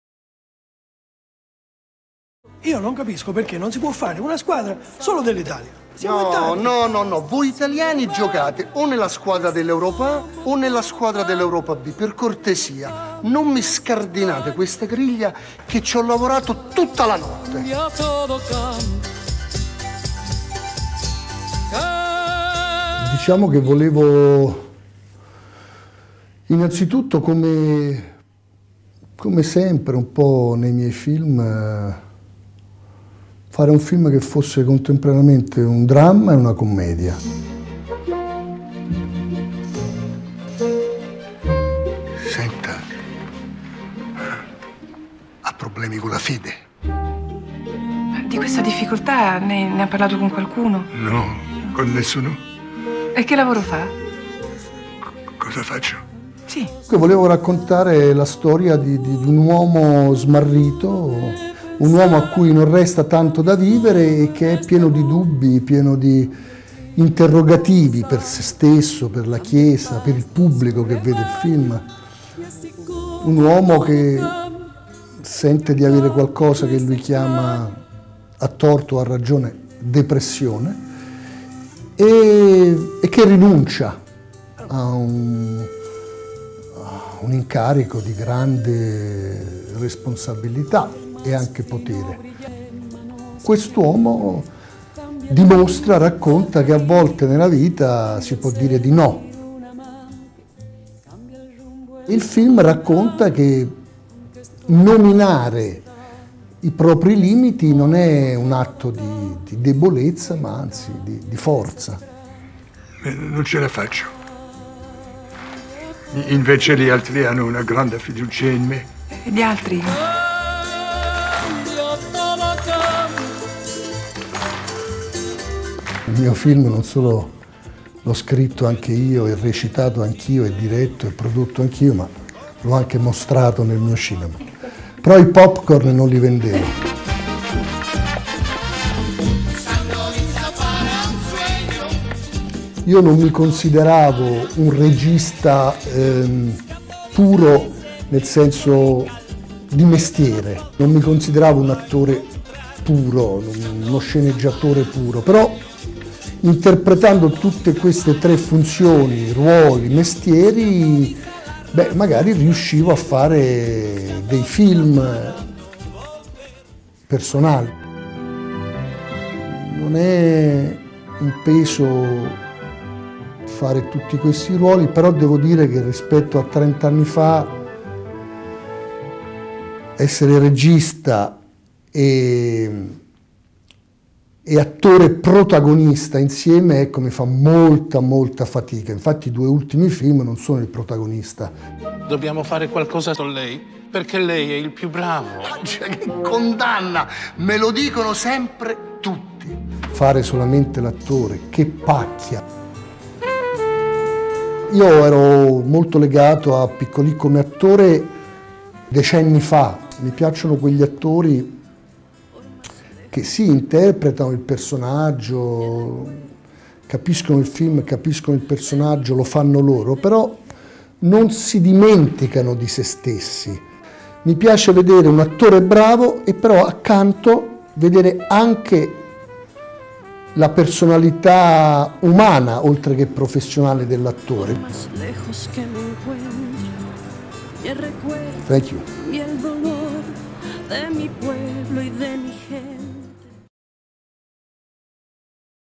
Nanni Moretti, an Interview With
Italian filmmaker Nanni Moretti is well known for his clever approach to poking fun at the pitfalls of his native country. His 2006 film, The Caiman, inspired by Silvio Berlusconi, of whom he is a staunch opponent, is said to have had an impact on the 2006 Italian general election. Crane.tv talks to the 58-year-old about his recent film, We Have a Pope, and about the increasing difficulty of merging the roles of actor, director and playwright.